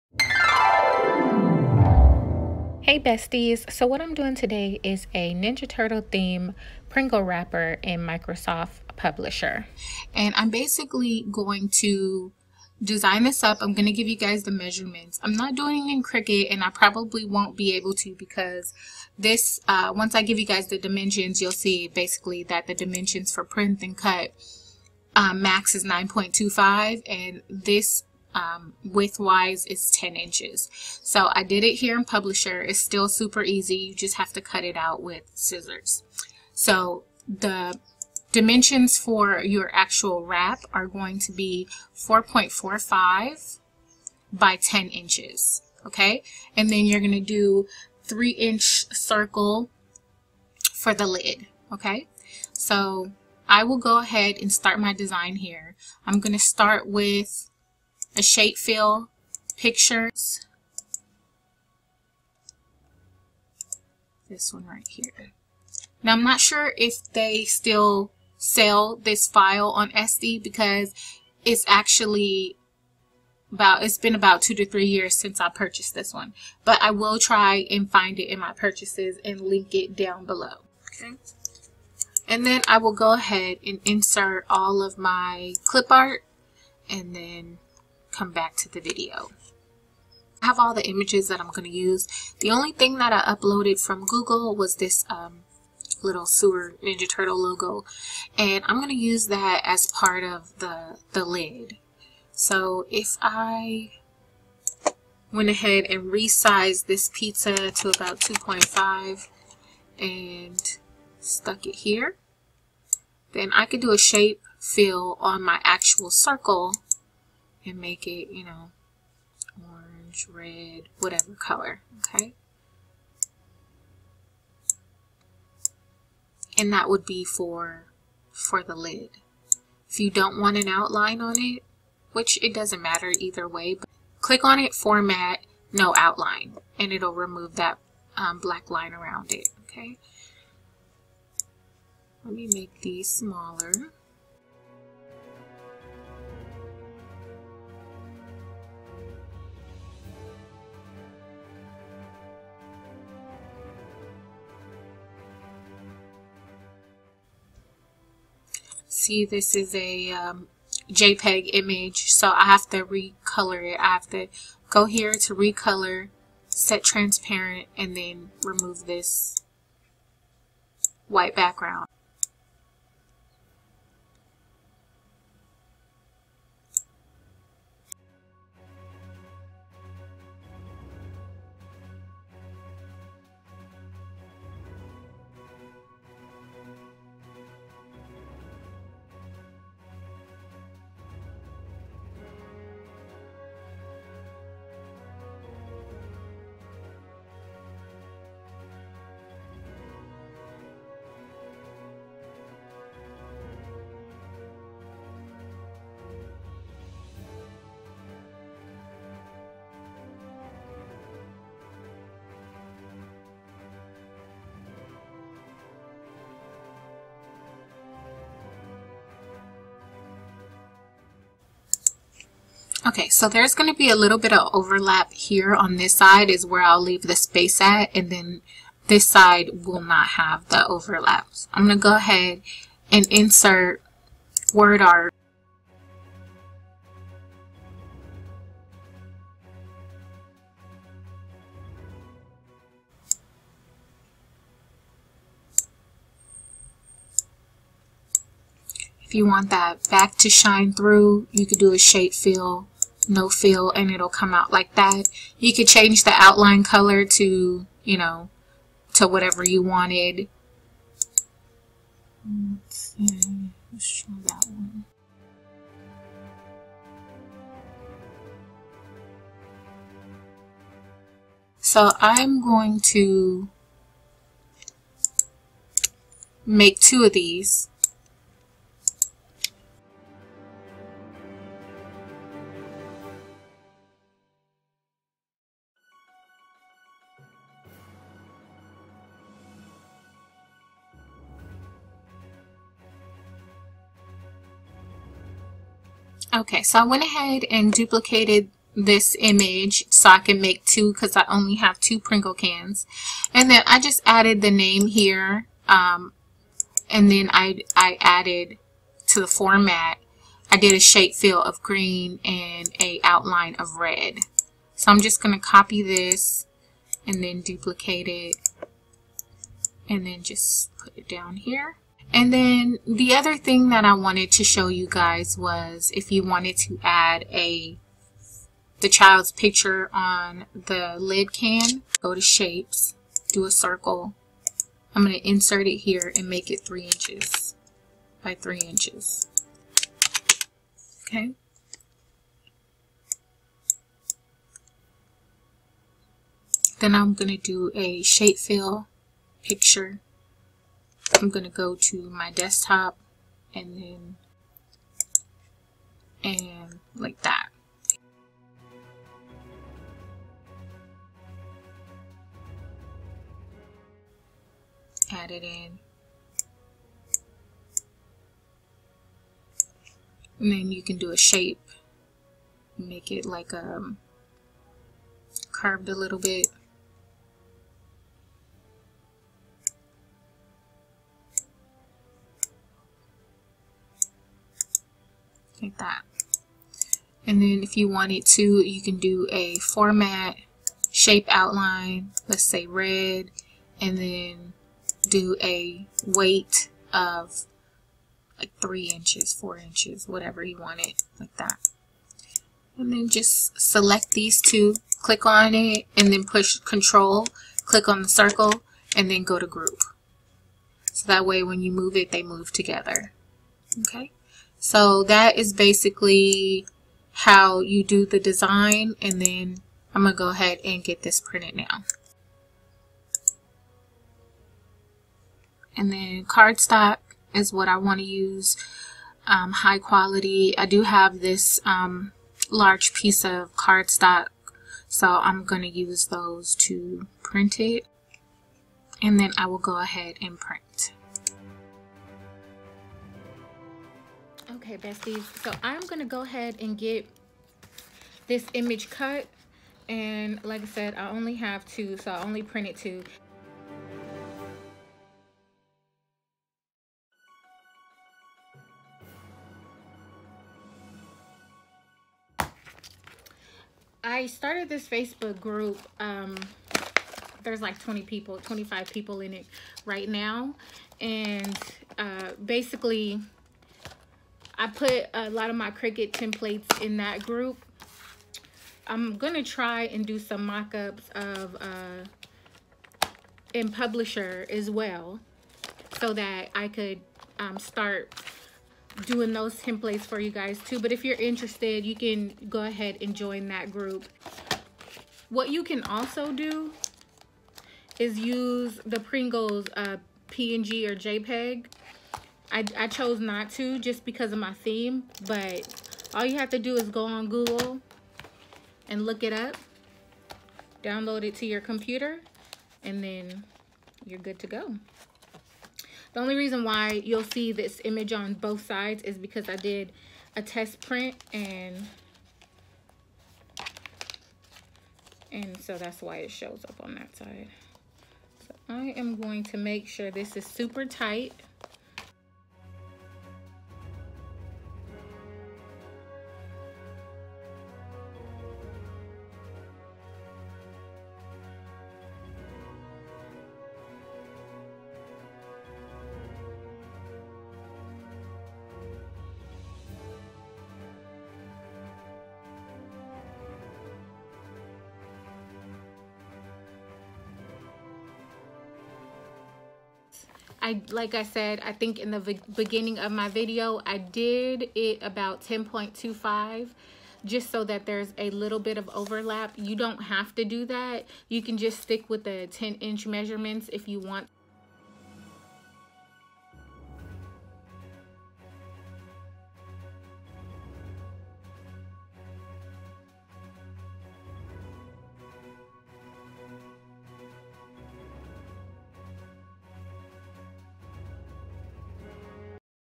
How to make personalized Pringles container with Microsoft Publisher l Tam's Sweet Life
Items used:

MS Publisher

Printer

Tape Dispenser